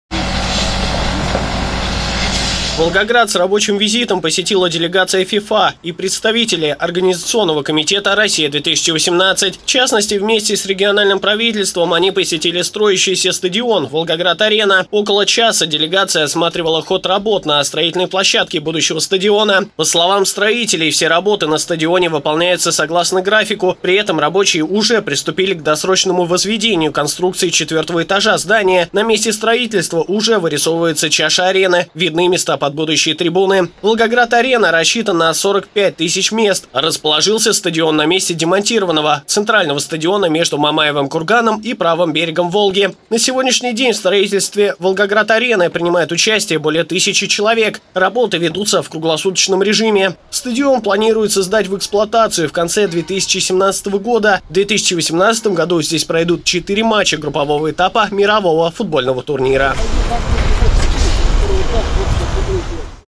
Делегация FIFA проверила спортивные объекты Волгограда
Волгоград с рабочим визитом посетила делегация FIFA и организационного комитета «Россия-2018».  В частности, вместе с региональным правительством высокие гости посетили строящийся стадион «Волгоград-Арена». Около часа делегация провела на строительной площадке будущего стадиона. По словам строителей, все работы на стадионе выполняются согласно графику, а некоторые идут с опережением. Рабочие уже приступили к досрочному возведению конструкций 4-го этажа здания, на месте строительства  уже вырисовывается чаша арены, видны места под будущие трибуны. «Волгоград-Арена» рассчитан на 45 тысяч мест, расположен стадион на месте демонтированного Центрального стадиона между Мамаевым курганом и правым берегом Волги. На сегодняшний день в строительстве «Волгоград-Арены» принимают участие более тысячи человек. Работы ведутся в круглосуточном режиме. Стадион планируется сдать в эксплуатацию в конце 2017 года. В 2018 году здесь пройдут четыре матча группового этапа мирового футбольного турнира.